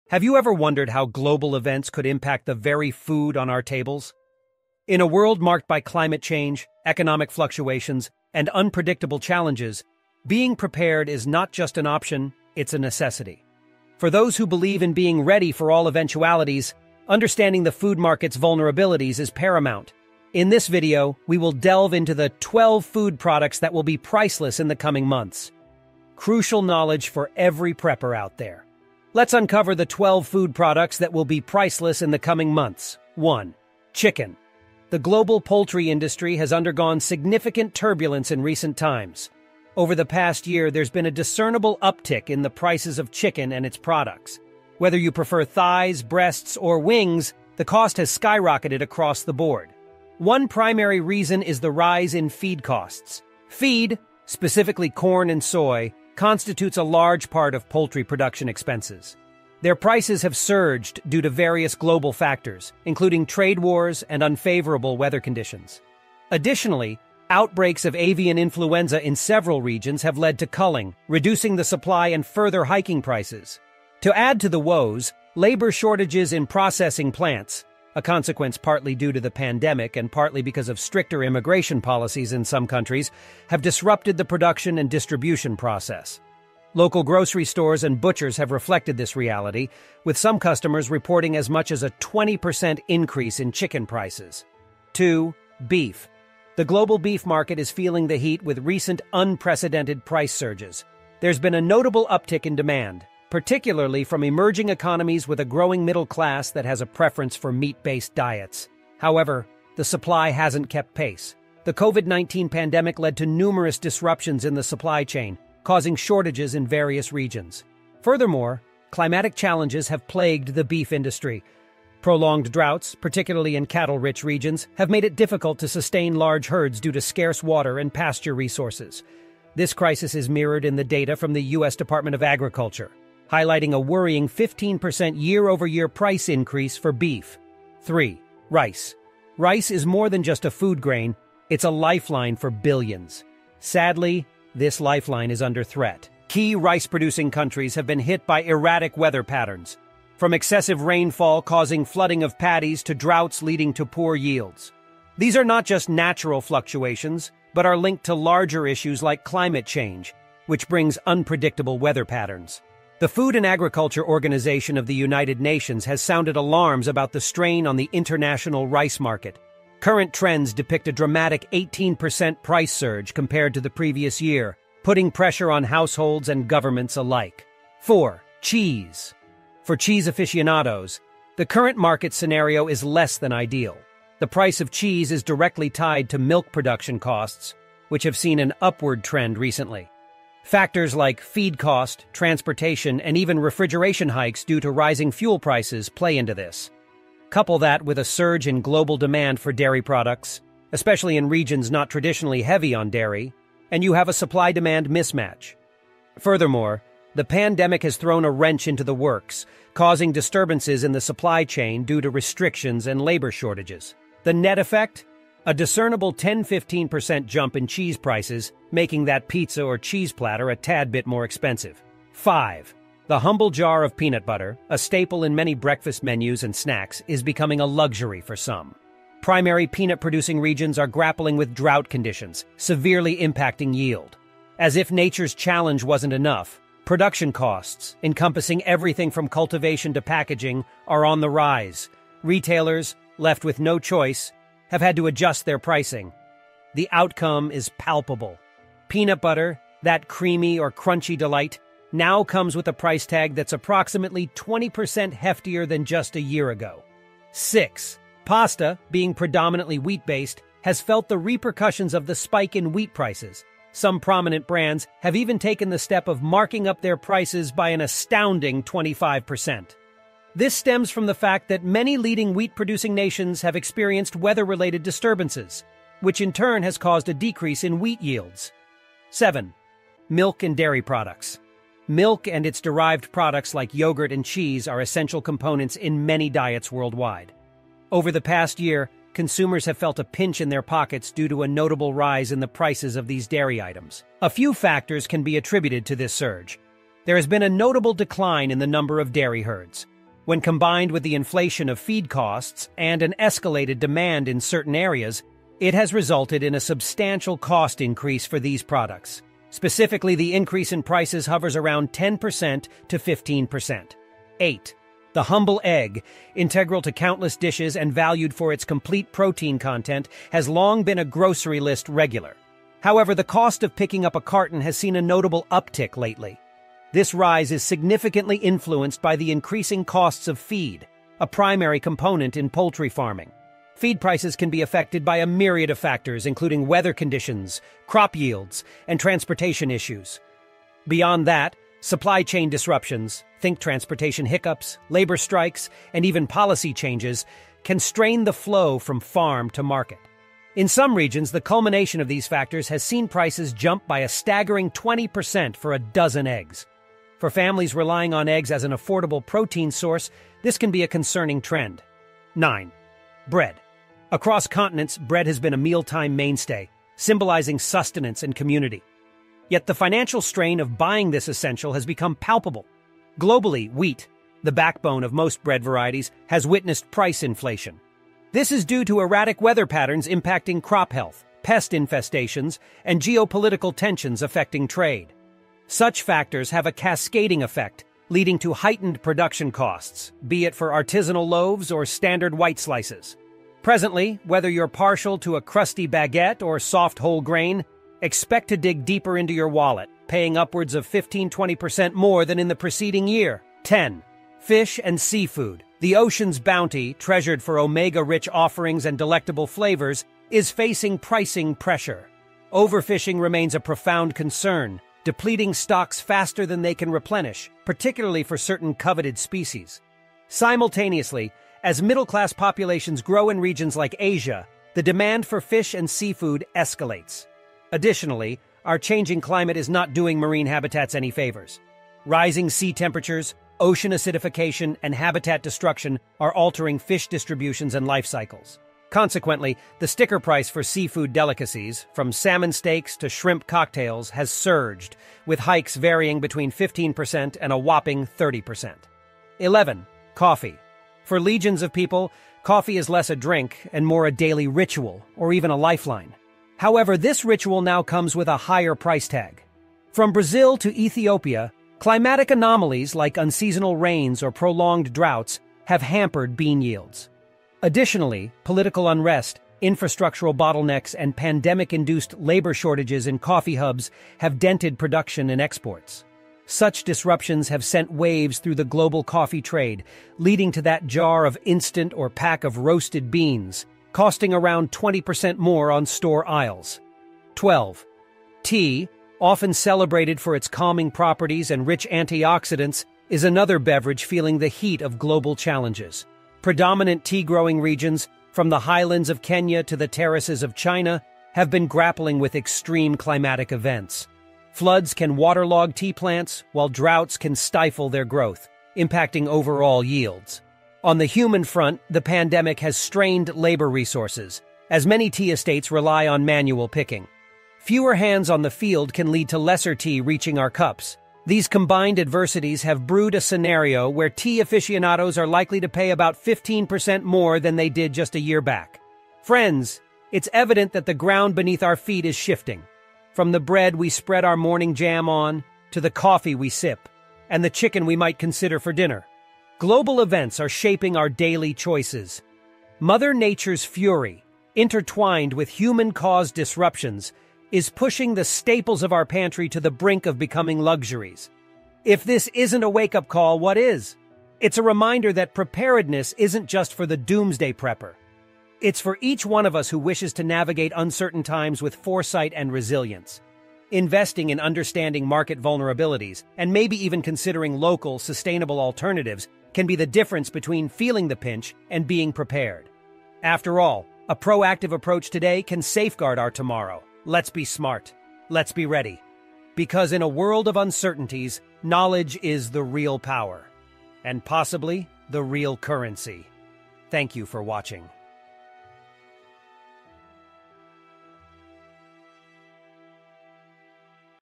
12 Food Products That Will Be Priceless In The Coming Months!
Have you ever wondered what would happen if our daily essentials suddenly became scarce? In an ever-changing world, being prepared is more than just a choice it's a necessity.

Welcome to our video on '12 Food Products That Will Be Priceless In The Coming Months!' where we'll dive deep into the current trends, global events, and disruptions that are set to reshape the prices and availability of some of your favorite foods. Grab a notepad, because the knowledge you'll gain today can be crucial for planning your future grocery lists!